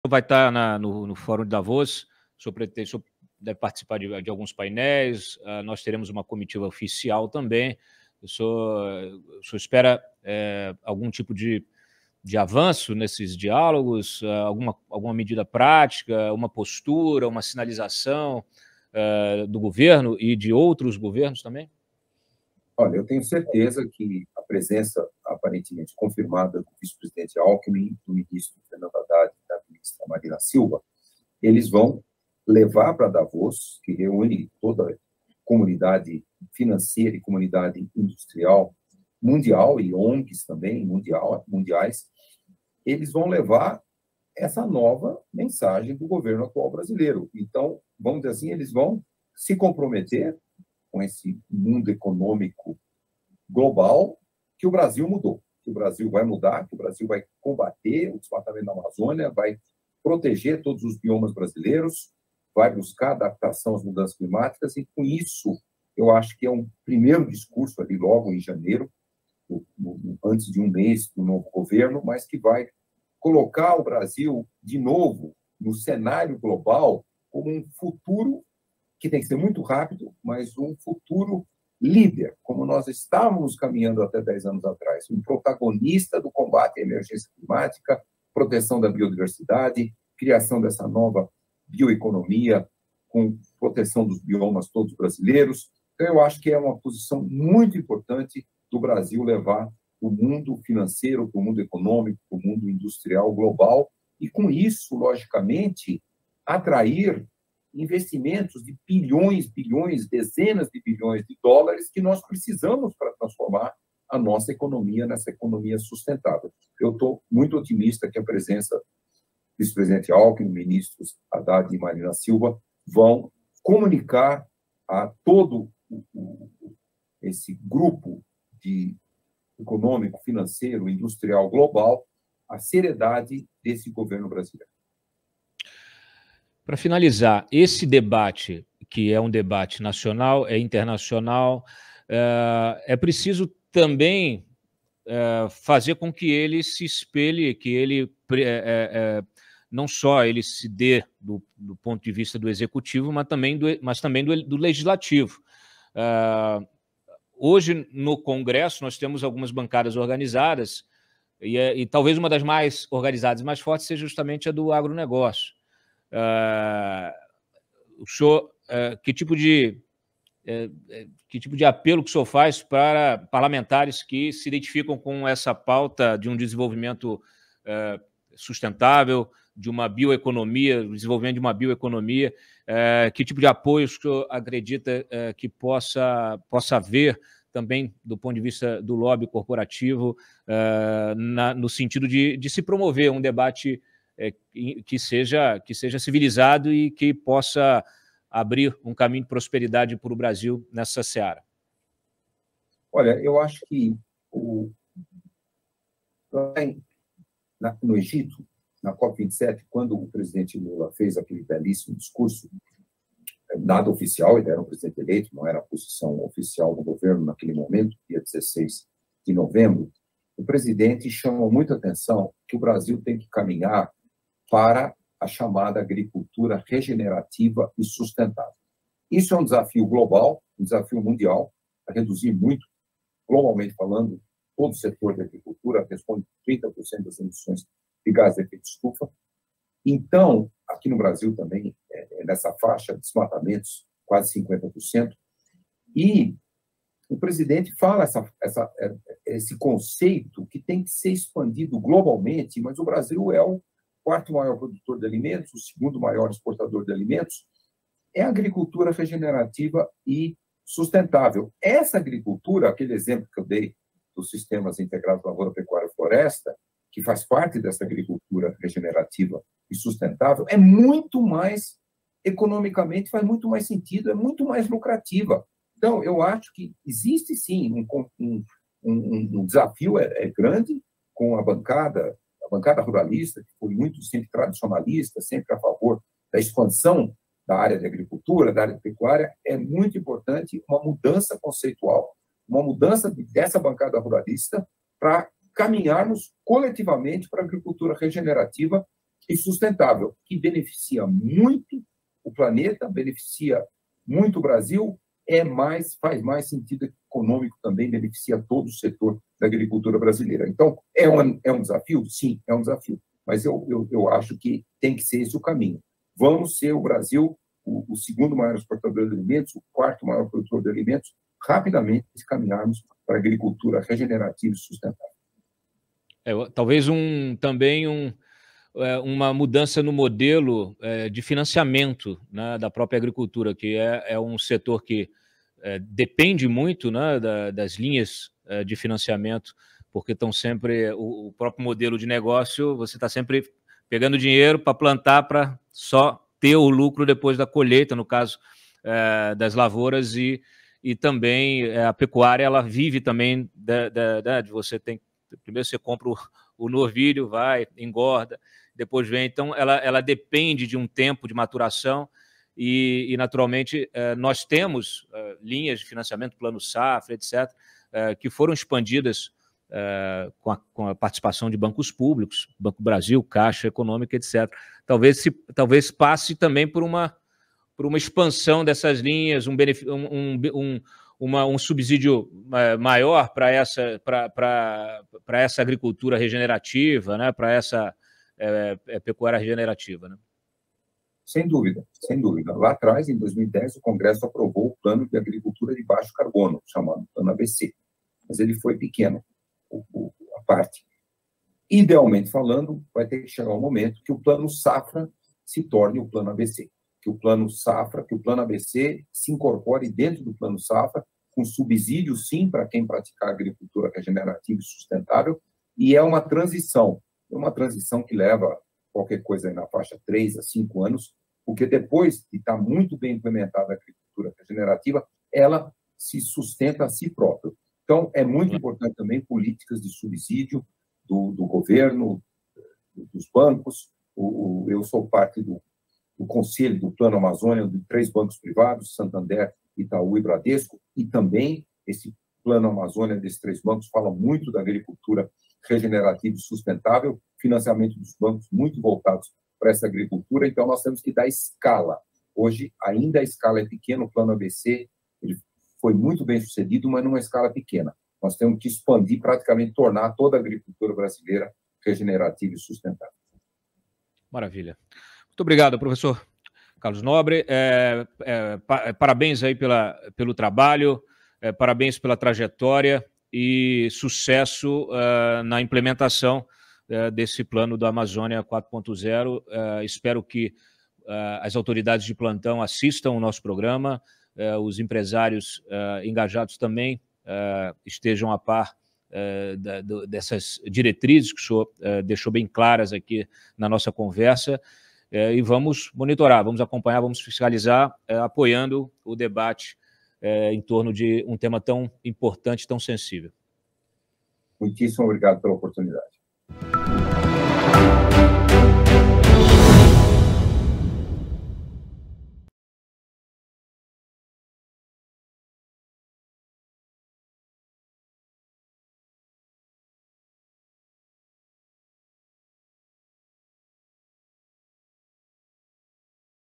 A bancada ruralista precisa mudar seus conceitos, alerta Carlos Nobre
O cientista fala sobre o futuro da floresta amazônica brasileira.